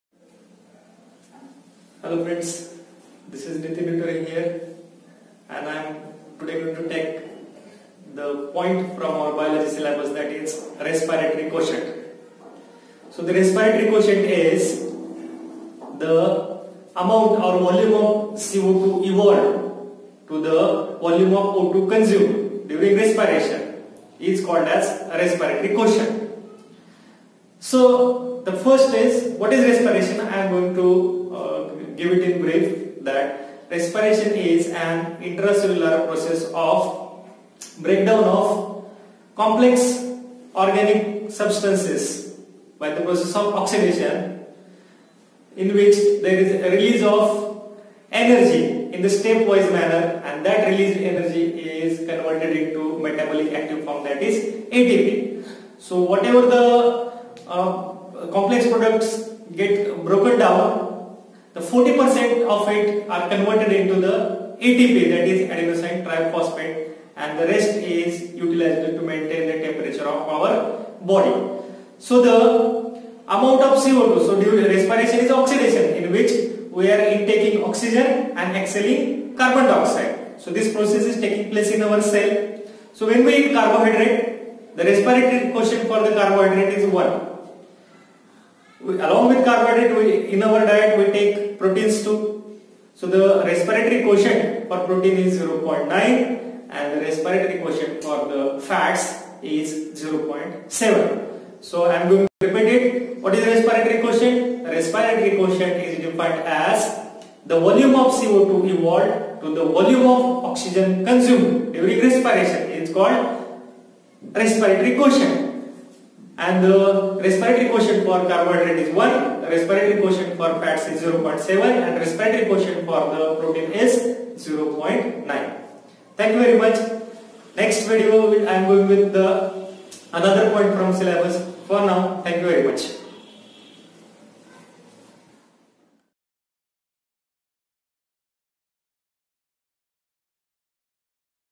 Respiratory Quotient
The ratio of CO2 evolved to the volume of O2 consumed in Respiration is called the respiratory Quotient or respiratory ratio.